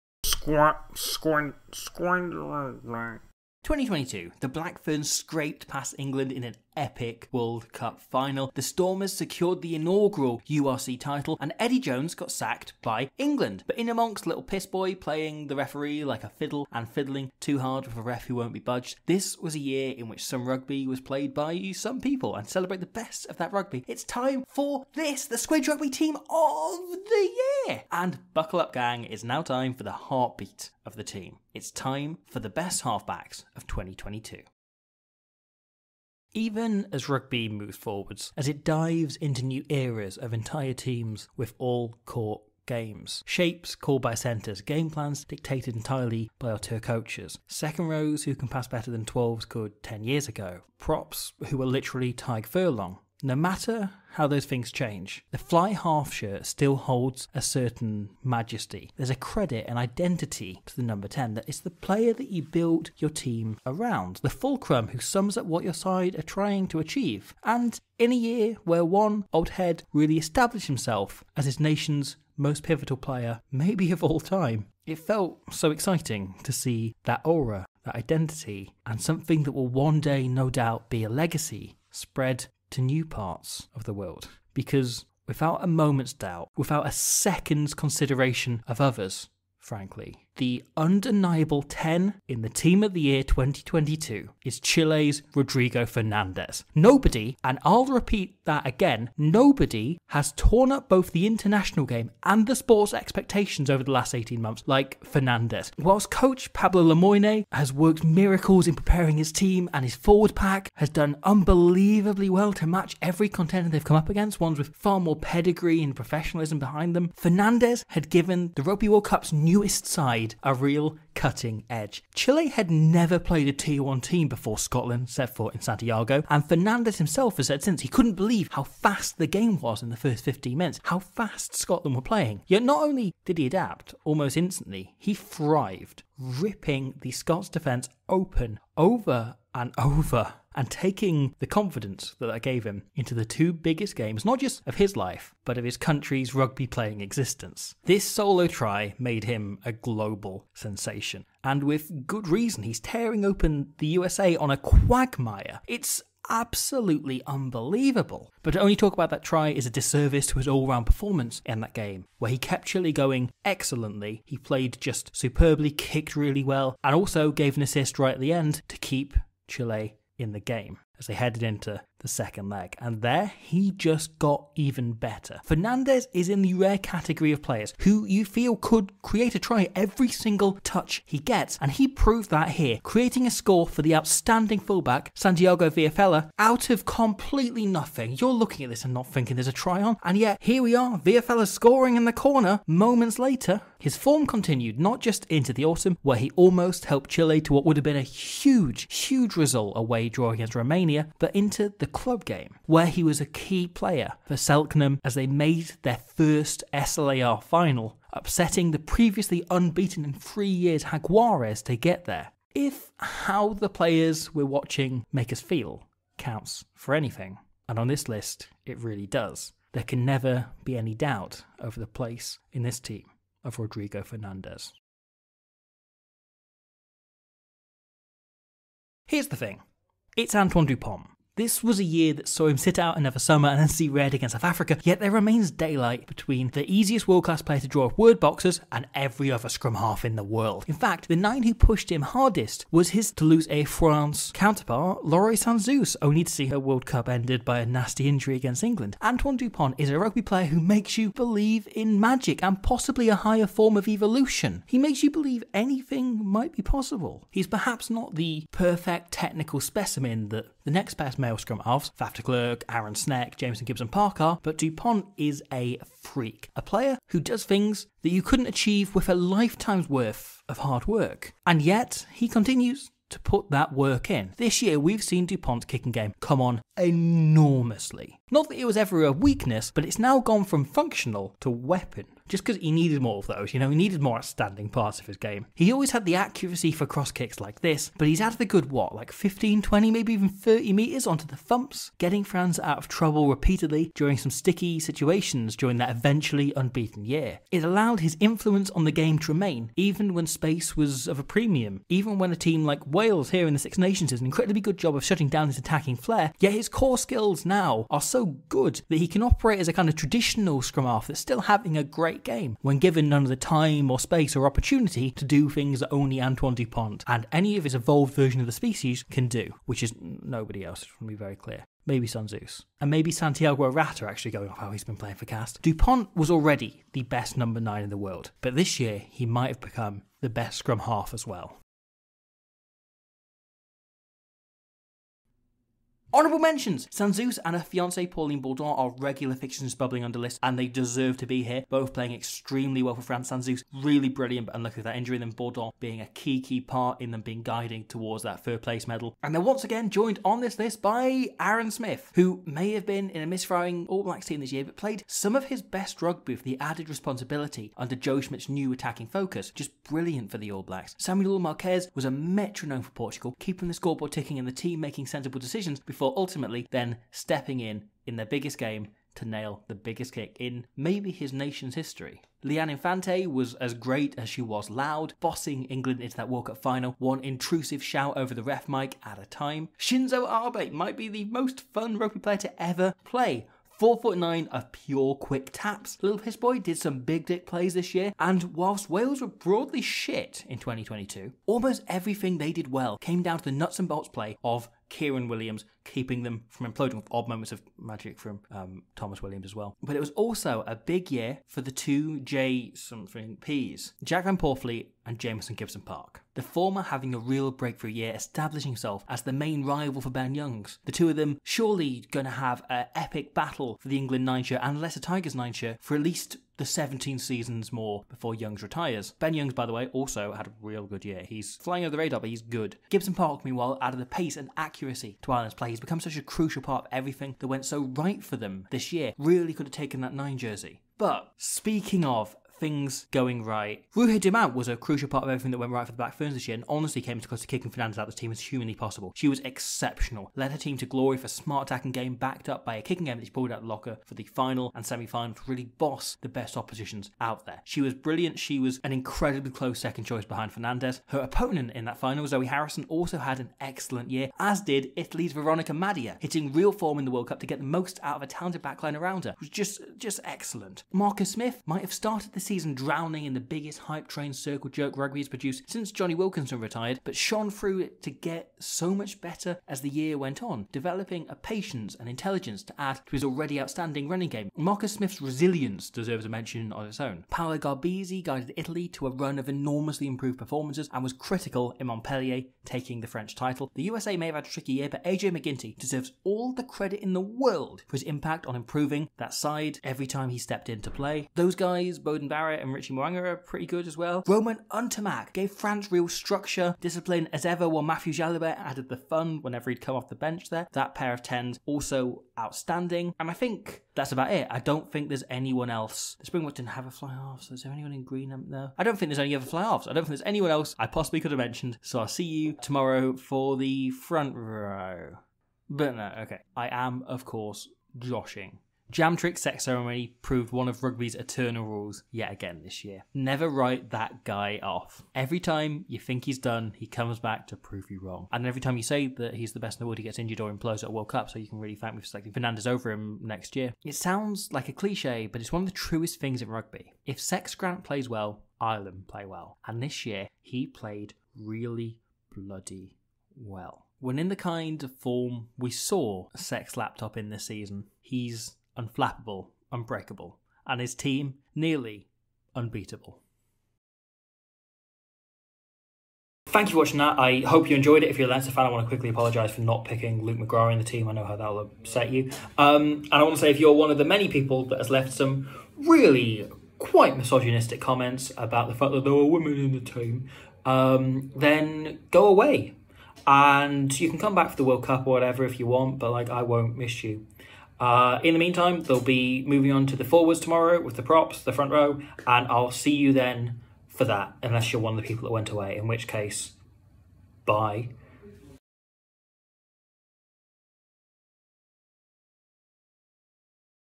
The Halfbacks | Team of the Year 2022
The heartbeat and brain. Last year, Ali Price controversially ousted Antoine Dupont for the nine shirt whilst Beauden Barrett bedded in at 10. Who will run the side for 2022?